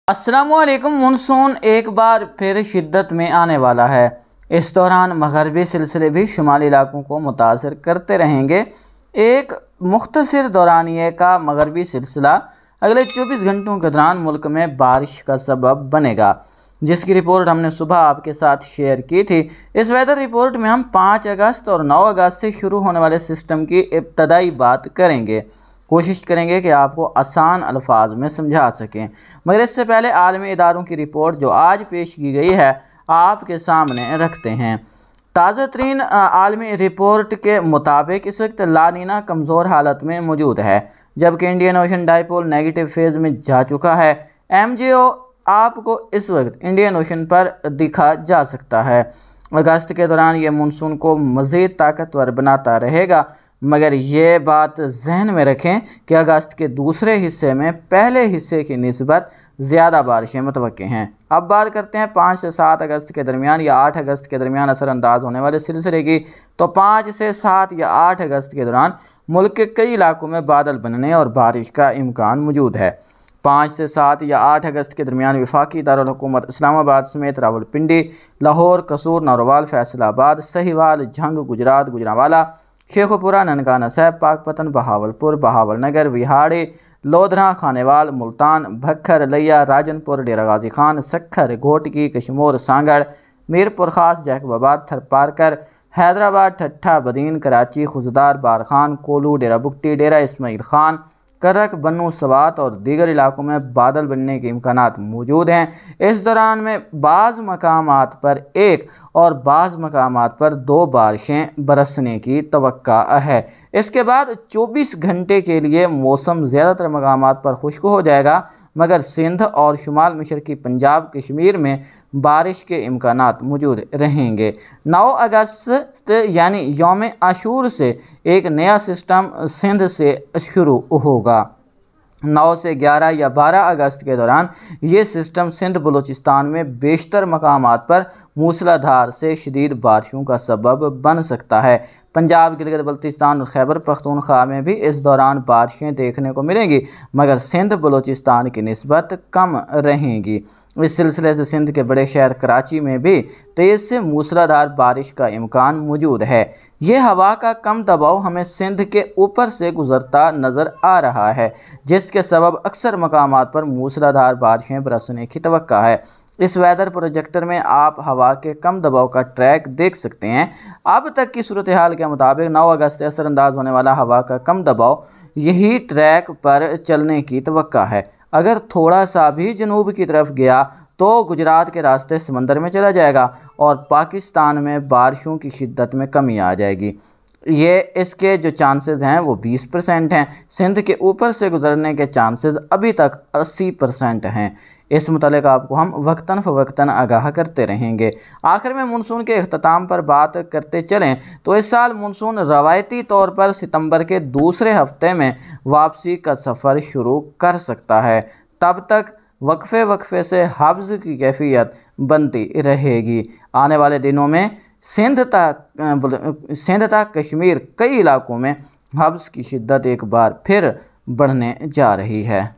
Monsoon Active Again In Pakistan | Torrential Rain Expected | Weather Forecast Pakistan | Mosam
Aslam o alaikum is video mein Pakistan Weather k New Rain Spell jo next rain spell hai monsoon update k sath paish kar rahy hain